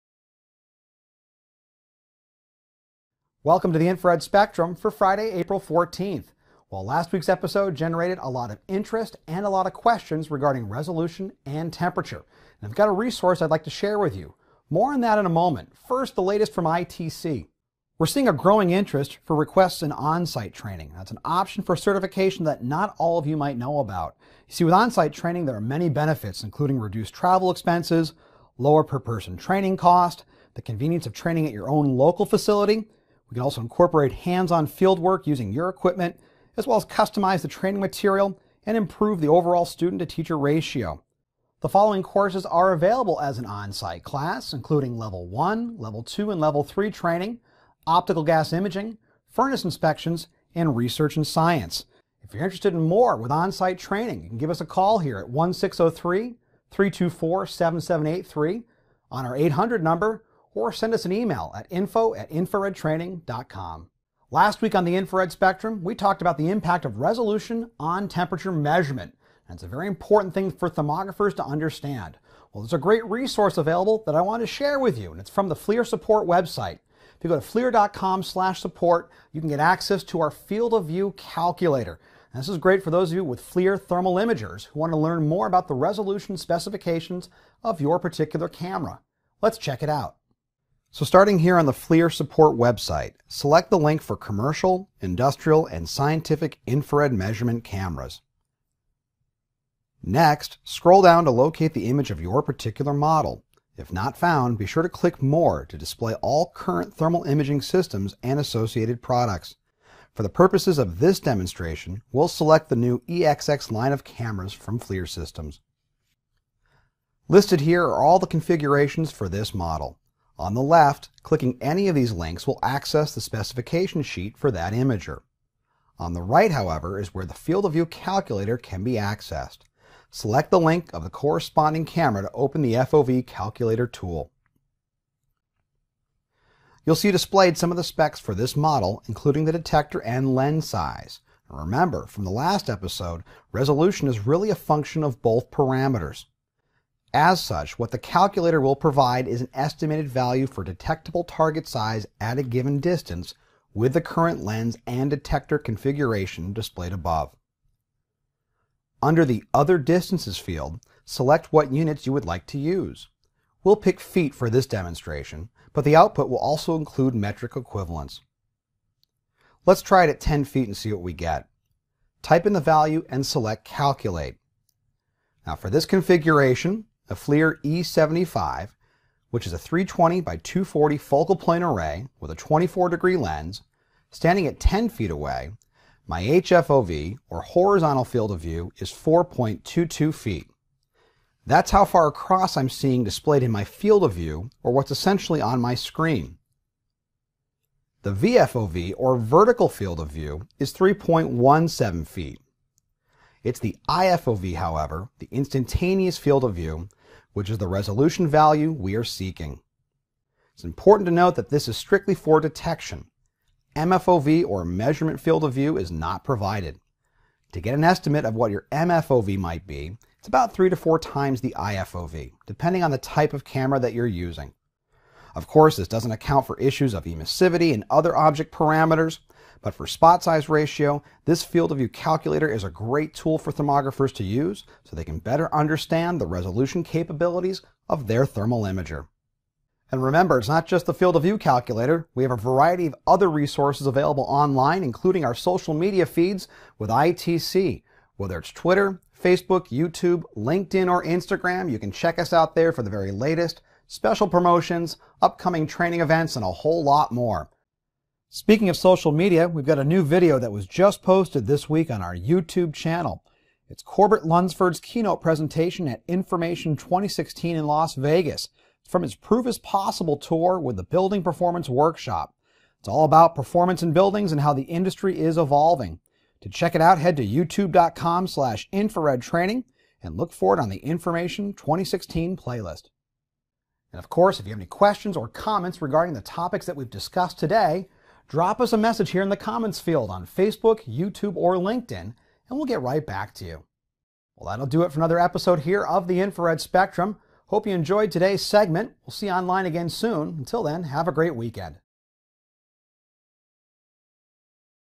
The Infrared Spectrum - Field of View Calculator
Ep6 - Field of View Calculator

Hosted by Matt Schwoegler, Infrared Training Center

The Field of View calculator is a great tool to use as a reference for those with FLIR thermal imagers that want to better understand the resolution capabilities of their camera.  In this week's segment we'll show you where you can access this tool, plus cover what's new at ITC.